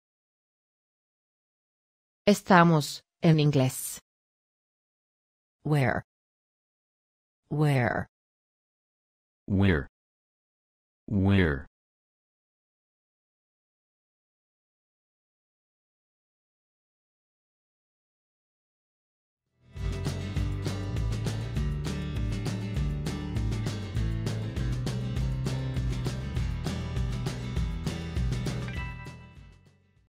Cómo decir "estamos" en inglés
¿Cómo se dice correctamente estamos en inglés? Escuche con nosotros.
¿Cuál es la traducción correcta de la palabra estamos del español al inglés? Habla como los americanos. Además, ¿cómo escribir correctamente la palabra we're?

¡Gracias!